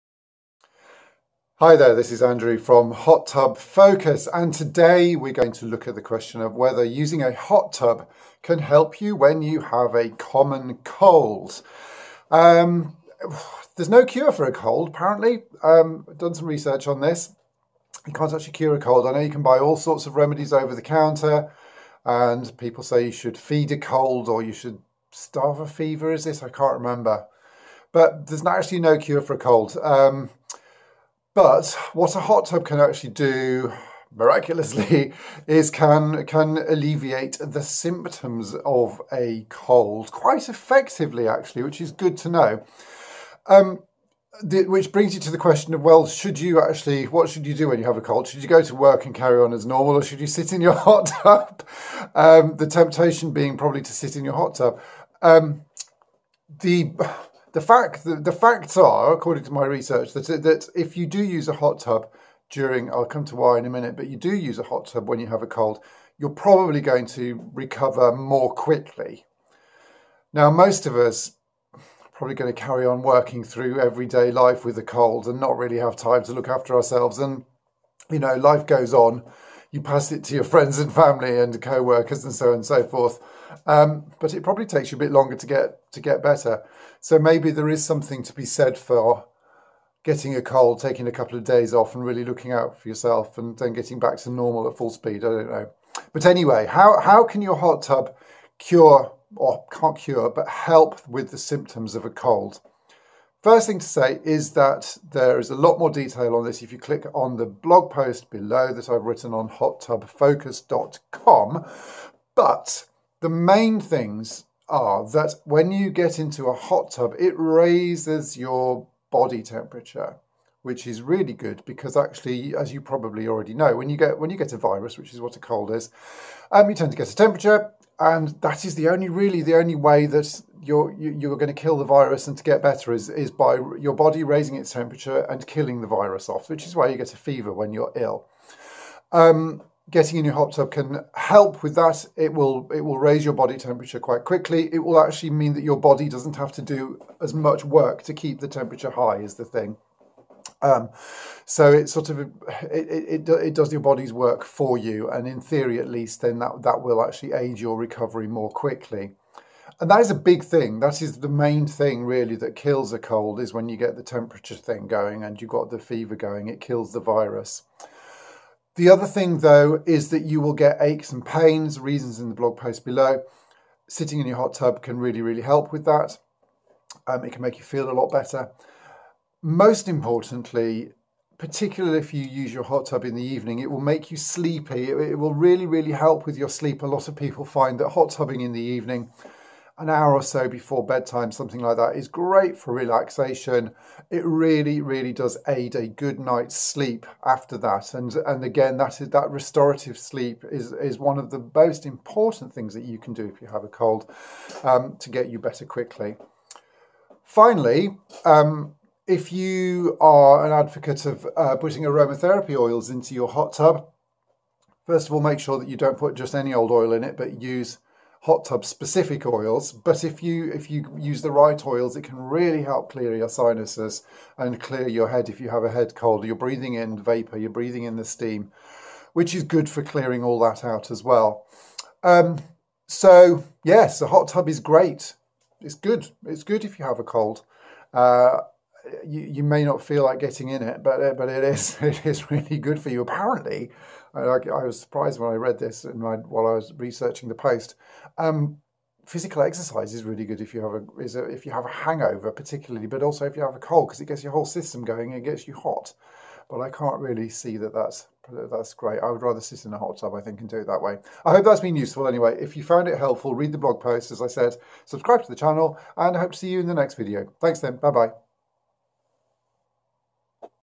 Does a Hot Tub Help a Cold?
Find out if using your hot tub when you have a cold is a good idea or not